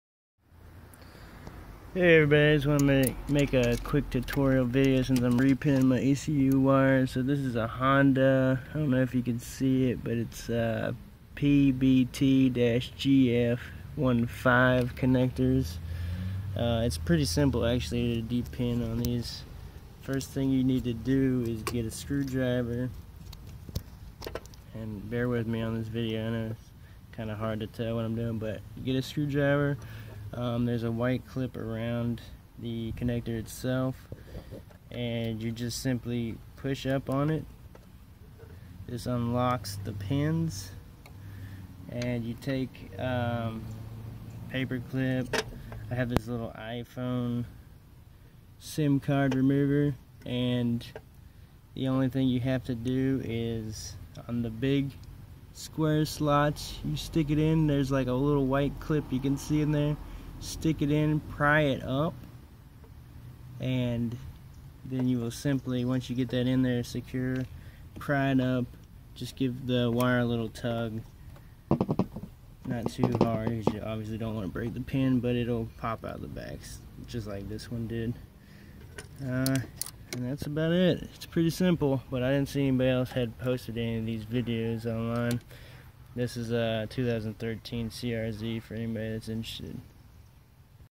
De-pin Honda ECU connector (PBT-GF15)
Quick video to explain removing the pins from a honda ecu connector.